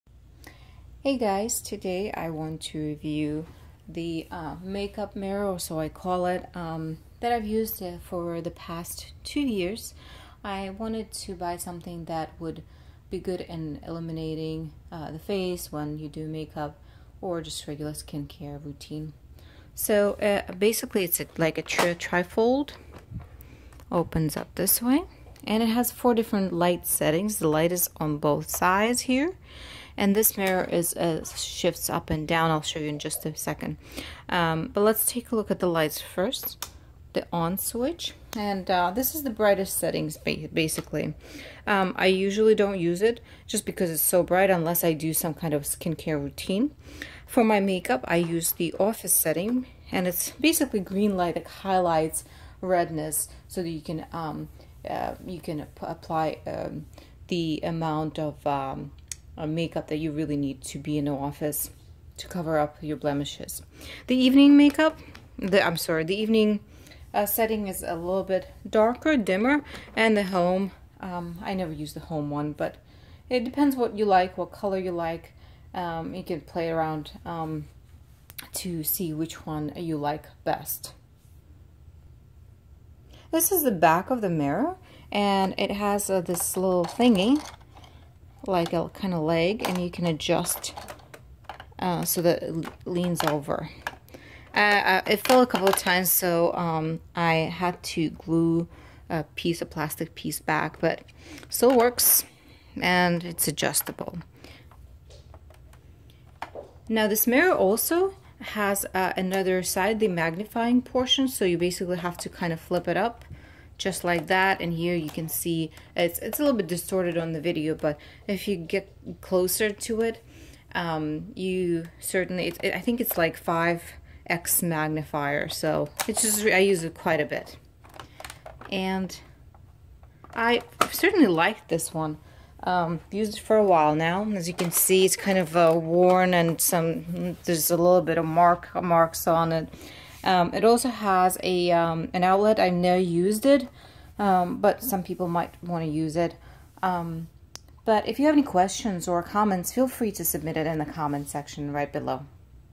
Video Review: Jerdon Tri-Fold Two-Sided Lighted Makeup Mirror with 5x Magnification
I said in the video that I have been using it for 2 years, but when I looked back at my order history on Amazon, I found out it's been 4 years. Time flies, I guess!